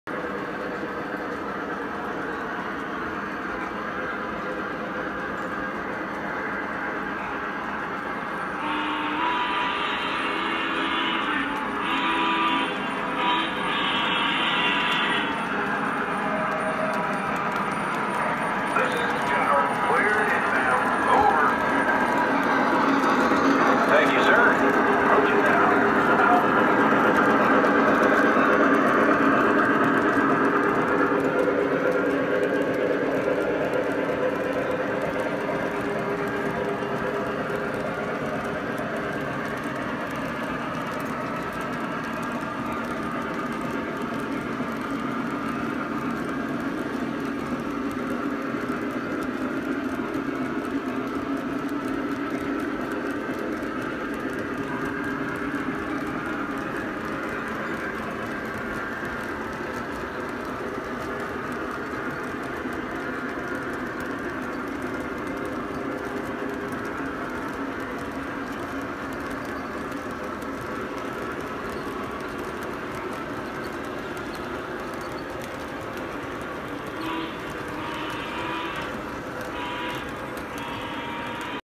40 Loads Of Coal On The Move
Lionel Legacy C&O 2-10-4 and H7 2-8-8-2 move 40 loads of coal.